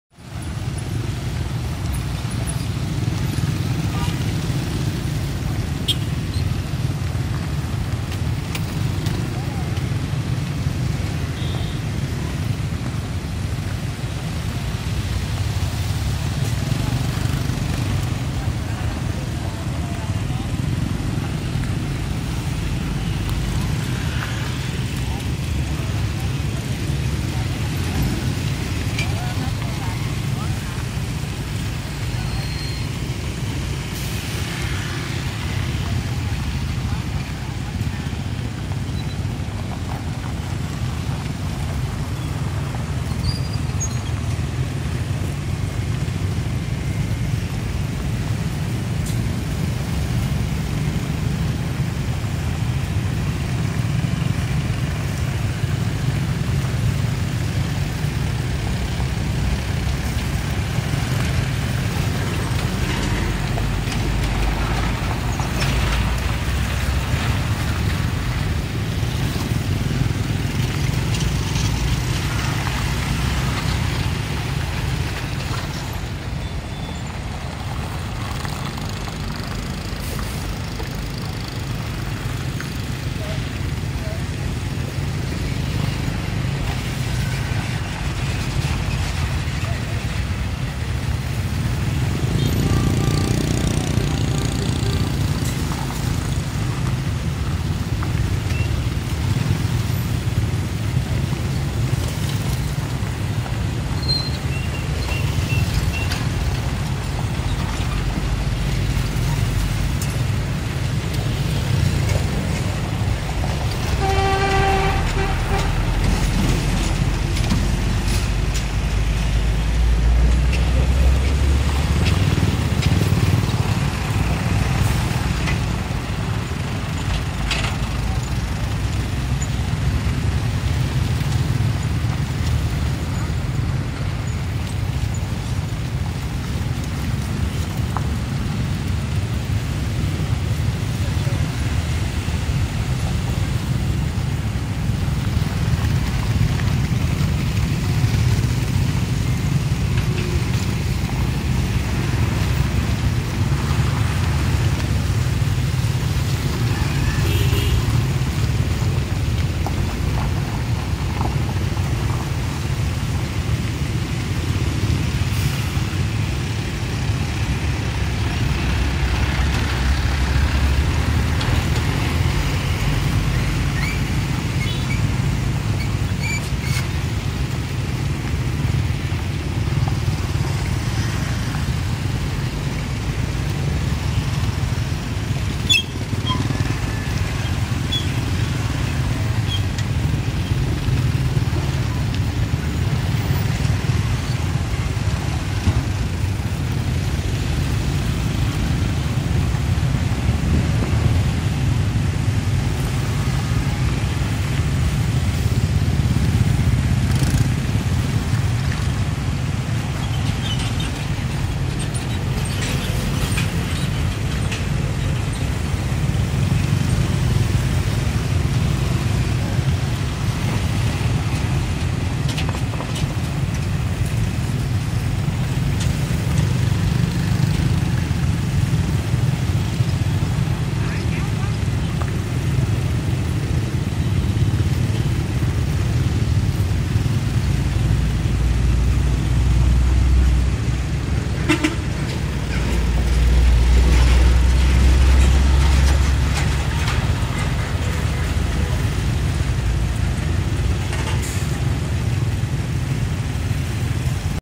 Công Trình Sủa Quốc Lộ 91 Dưới Cầu Mương Trâu Một Chút - Phần 2
Quay dưới Cầu Mương Trâu chút trên Quốc Lộ 91
2022.10:12 16:41 An
Phạm vi công cộng